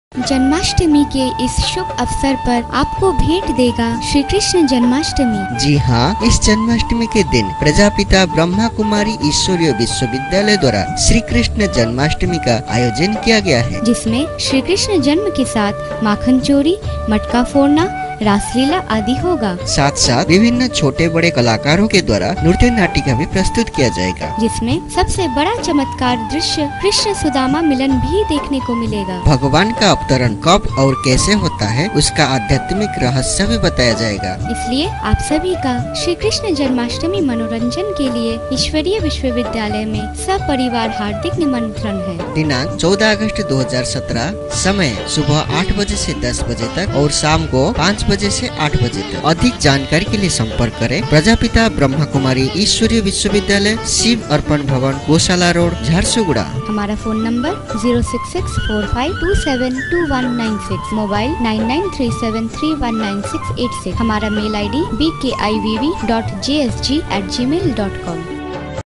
Janmastami Advertising
Janmastami Advertising in Brahmakumaris, Jharsuguda, Odisha on 14th aug 2017.